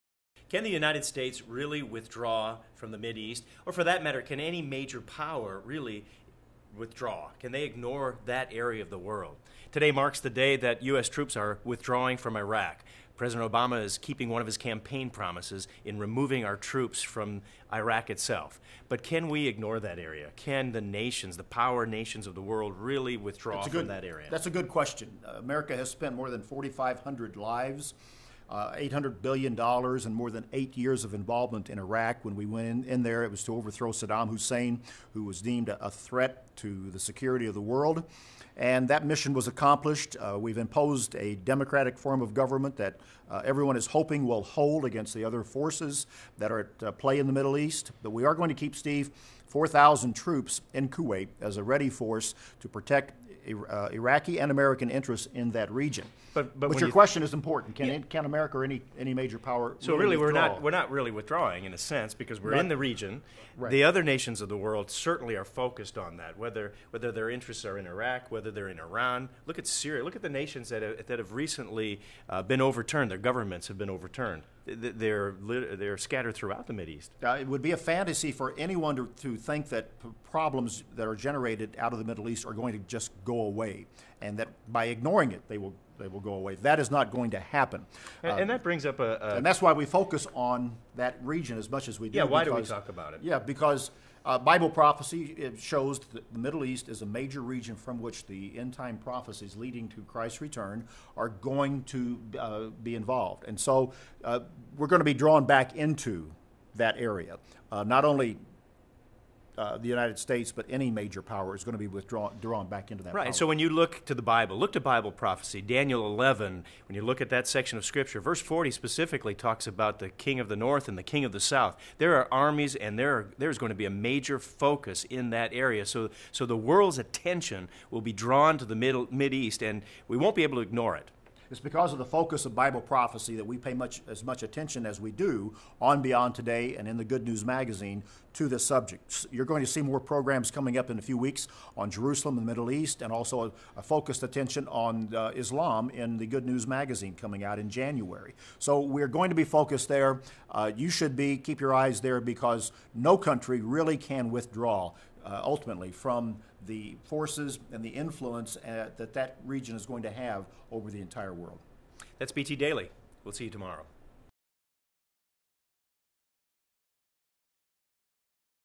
BT Daily: America Pulls Out of Iraq
December 15, 2011 - But will any major power ever completely withdraw from the Middle East? Bible Prophecy shows the answer.